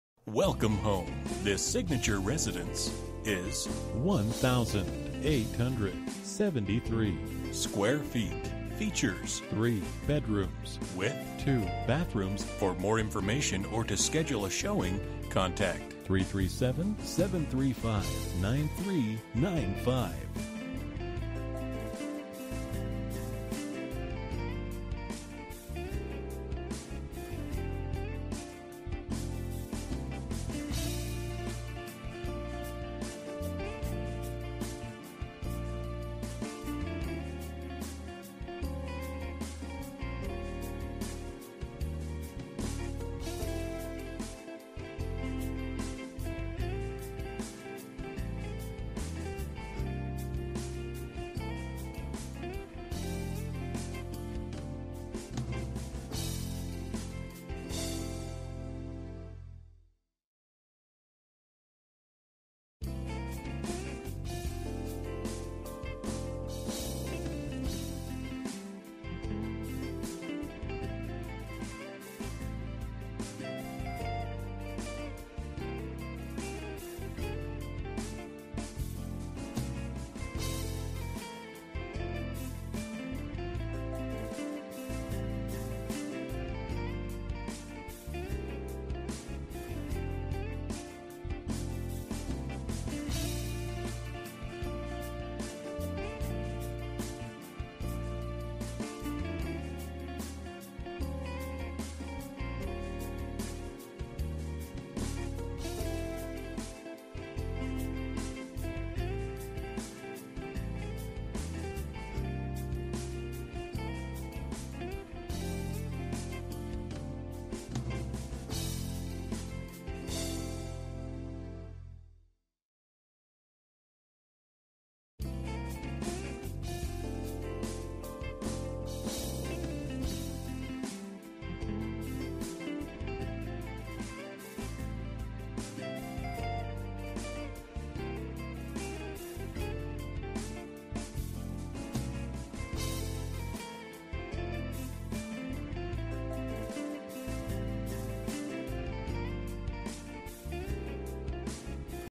Residential for sale - 209 Chevalier Boulevard, Lafayette, LA 70503
Beautiful corner lot home in a nice subdivision. This is a 3 bed/2 bath home located in Lafayette. Amazing open floor plan with plenty of space to entertain friends and family. Master bedroom offers a very spacious closet. Kitchen offers granite countertops. Computer nook that can be used as an office area. Great backyard with a covered patio. This is must see! OWNER/AGENT

Bedrooms: 3
Bathrooms: 2
Square Feet: 1873
Price: $220,000

03/11/2019 05:56:35 am

Last modified: